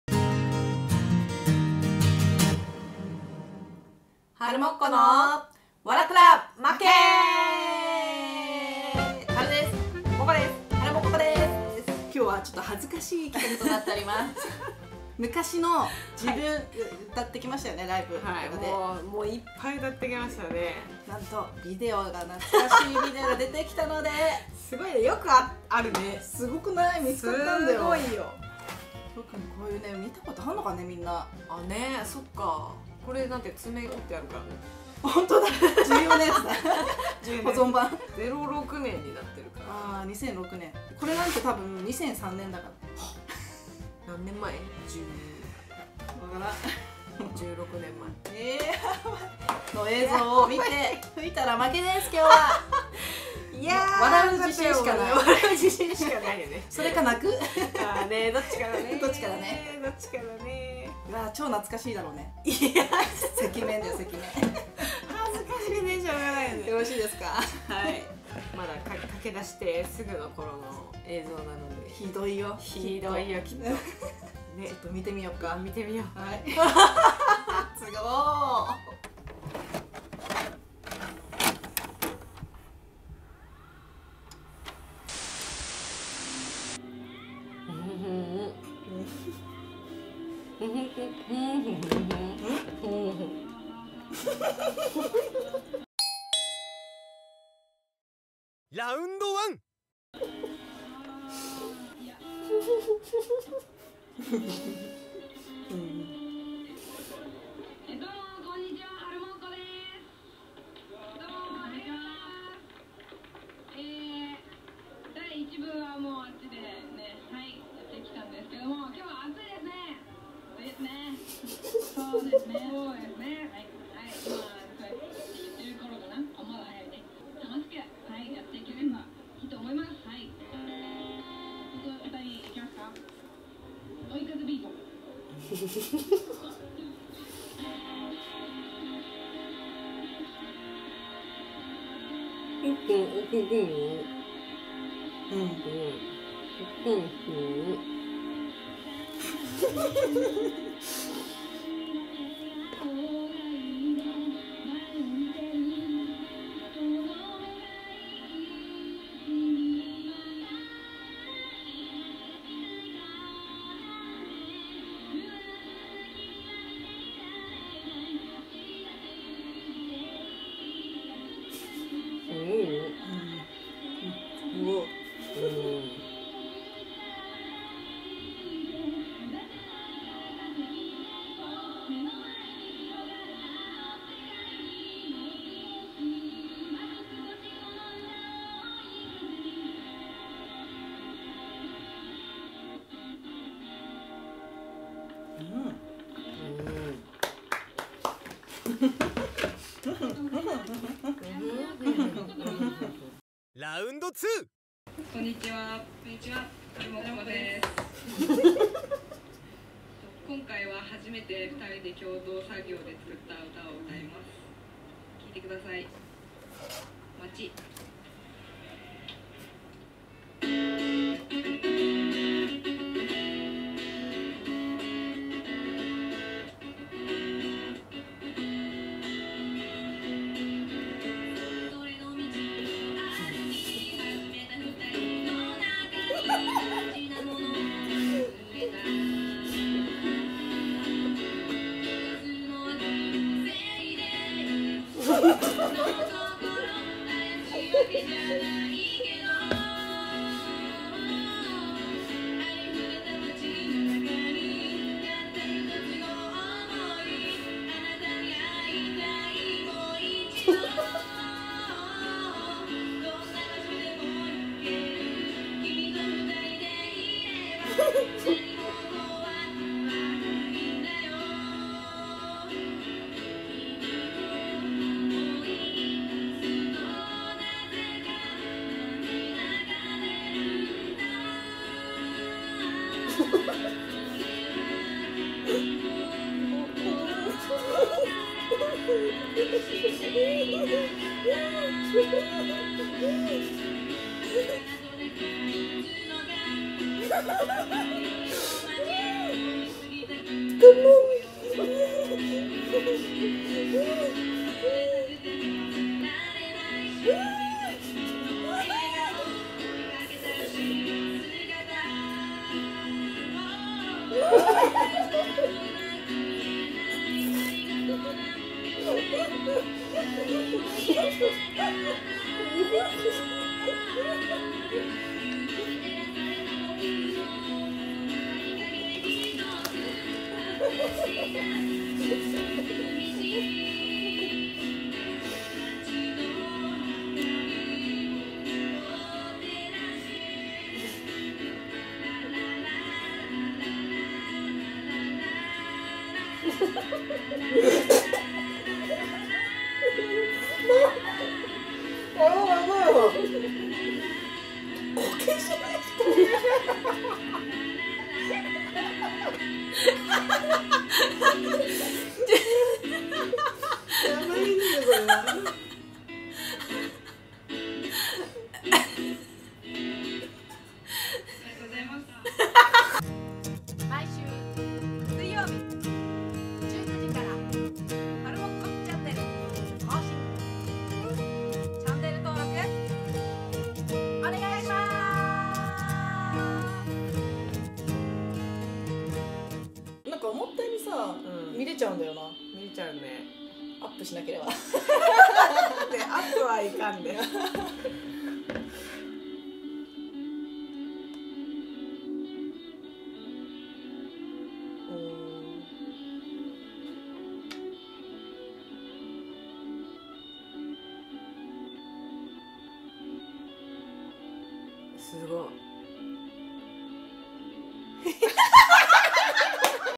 昔の自分に笑ったら負け！ビデオテープを恐る恐る見てみた！はるもっこ☆
シンガーソングライターユニット「はるもっこ☆」
毎週水曜日１９時更新中
はるもっこ☆HP